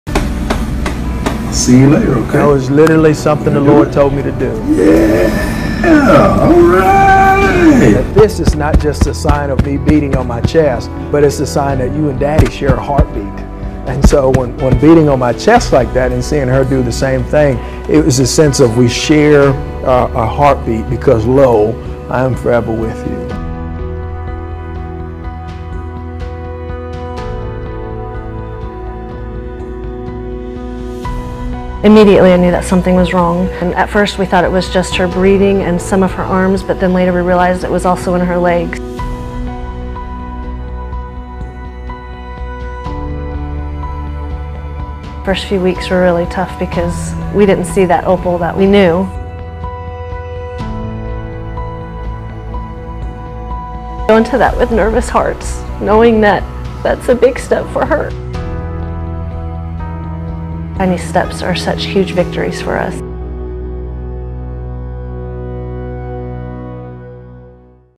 Father was ‘sharing a heartbeat’ with daughter battling rare illness in viral video
A father of a girl battling a rare illness explains what he and his daughter were doing in a heartwarming video that has since gone viral.